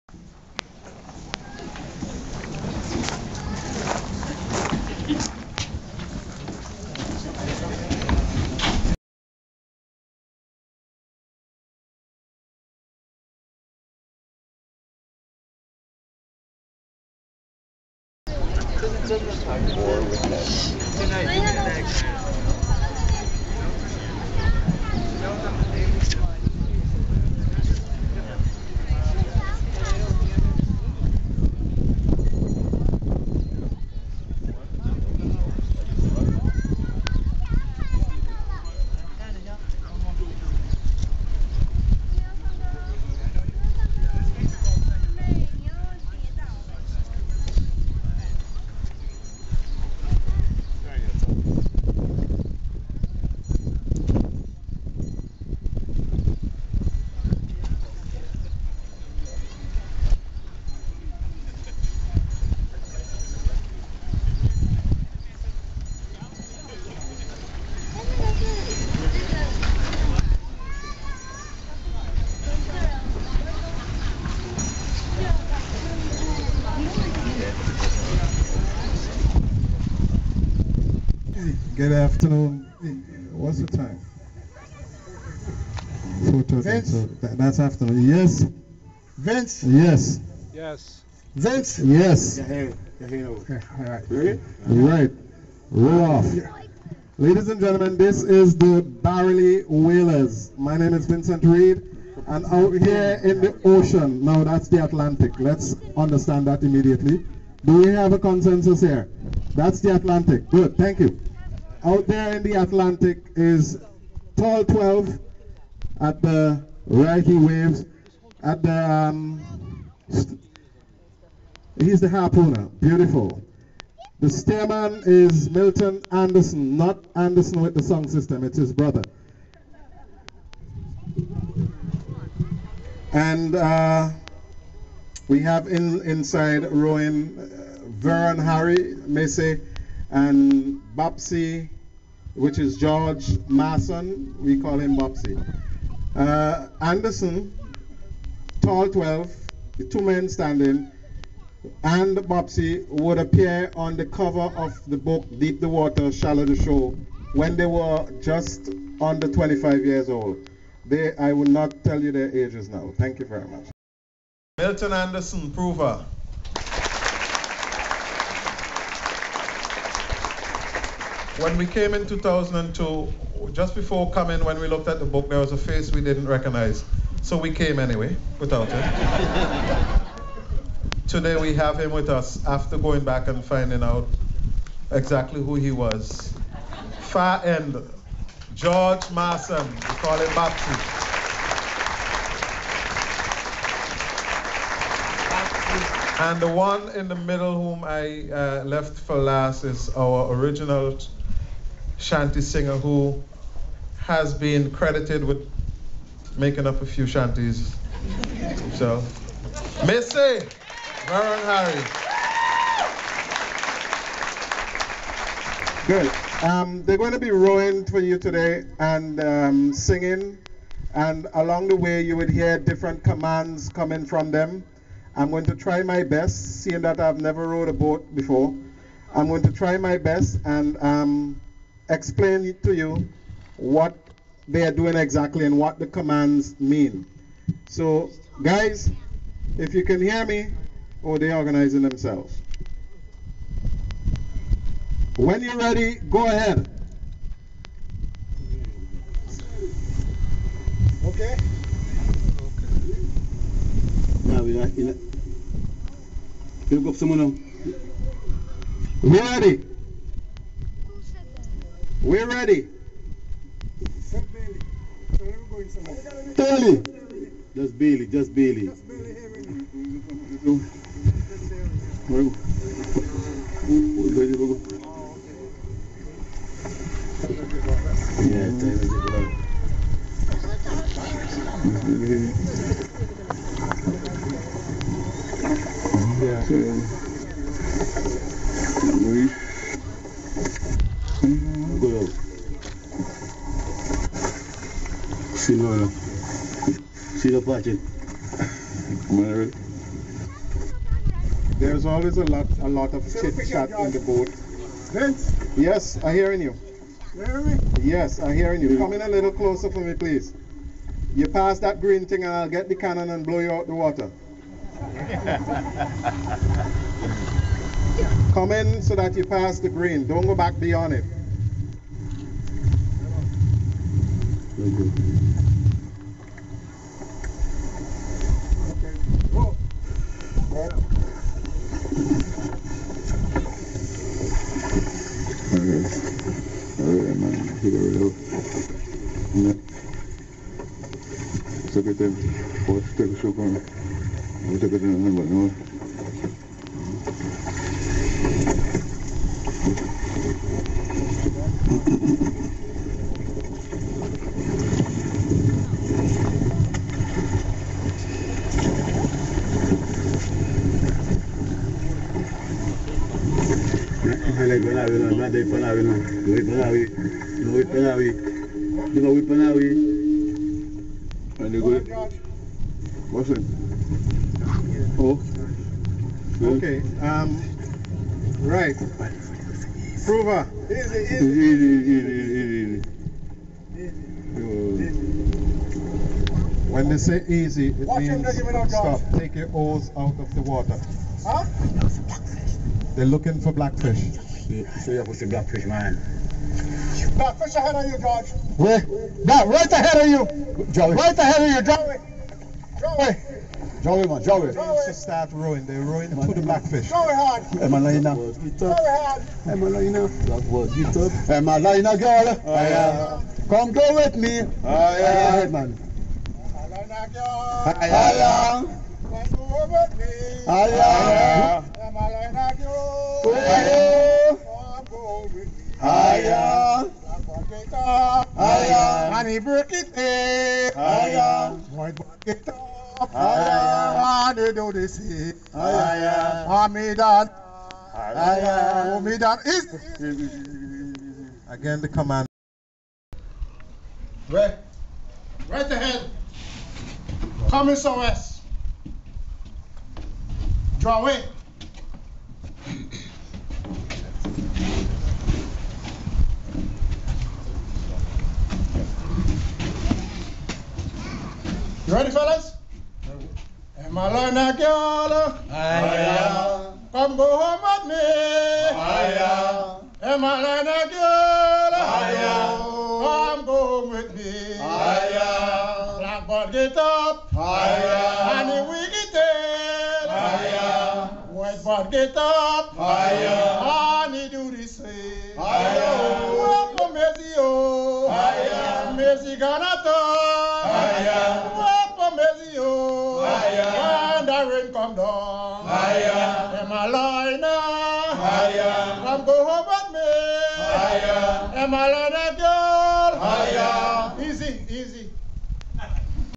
Barrouallie Whalers - Whaleboat Demo, Shanties, & Songs (pt. 1 of 4)
In Mystic, Connecticut.  Friday June 11, 2010.